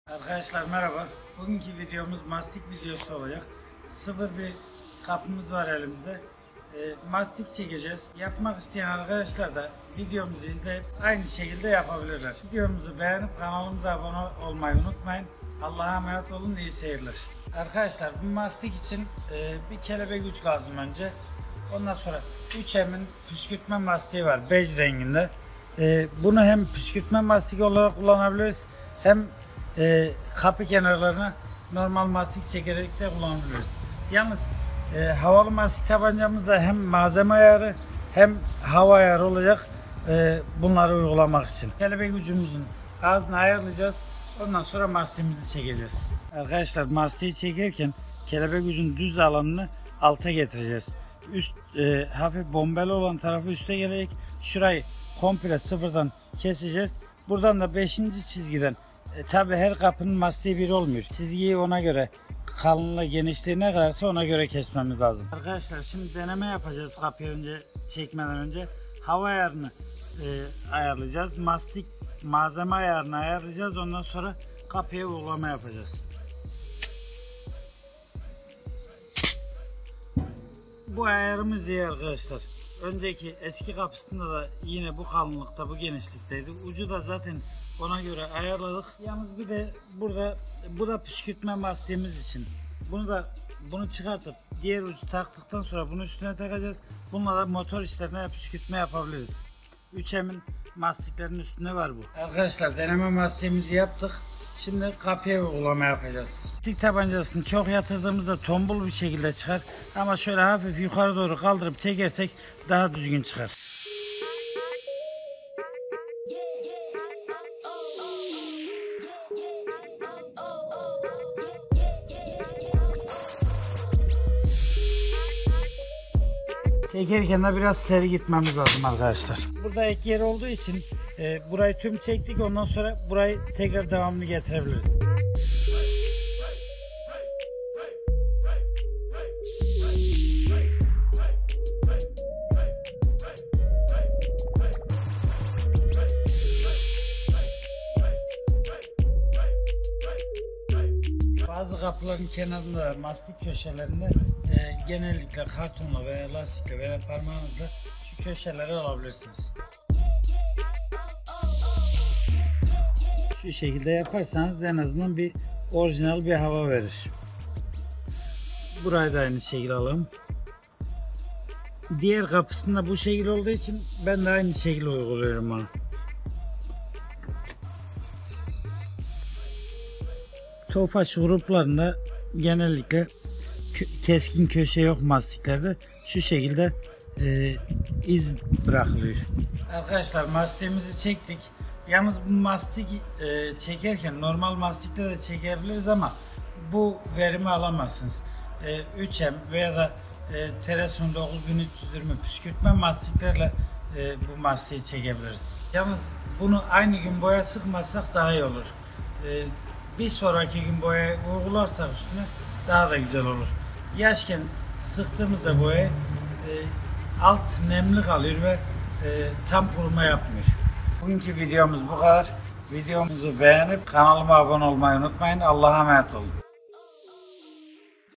ARABALARA MASTİK SİLİKON NASIL ÇEKİLİR? 3M PÜSKÜRTME MASTİK İLE SIFIR KAPIYA MASTİK ÇEKME
Mastik ve pütür sürecinde en çok değer verilen özellik, fabrika çıkışı uygulamaya olabildiğince yakın sonuç elde etmektir. Bizde sizlere en iyi şekilde mastik nasıl çekilir onu anlatmaya çalıştık.